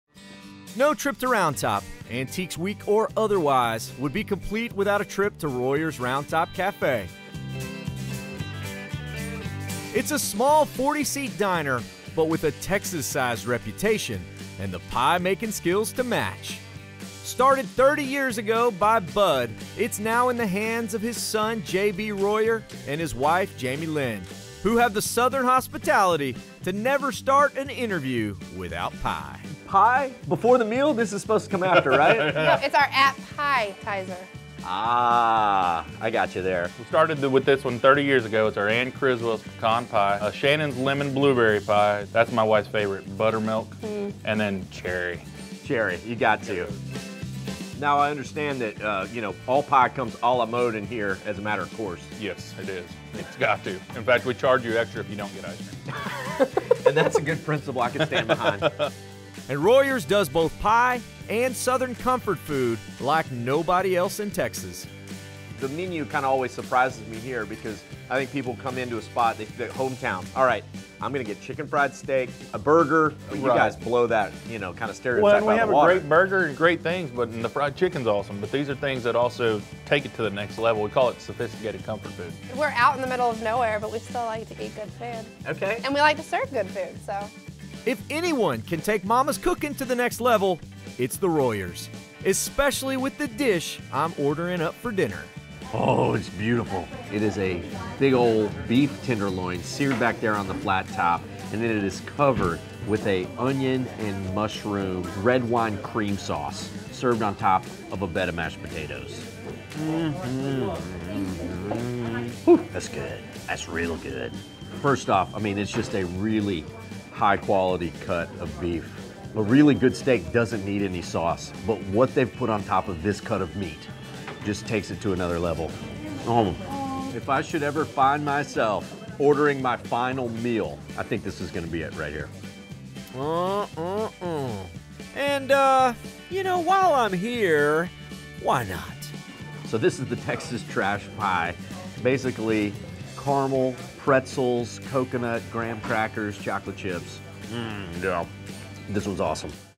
Royers Round Top Cafe: Best Pie + Comfort Food in Texas?
Round Top is known for Antiques Week, but the Texas Trash pie from Royers Round Top Cafe is where it's at. Nothing hits the spot after a day of shopping in the Texas flea markets like a slice of Royers' Round Top Pie and a plate of classic southern "sophisticated" comfort food.

Round Top is a land of antiques, Junk Gypsy fun and plenty of pie. Stop into this local favorite Texas cafe for good eats after a long day and decide for yourself if this is the best diner in Texas!

And watch the FULL Round Top, TX episode where we hang out with the Junk Gypsy gals for a little junk-tripper fun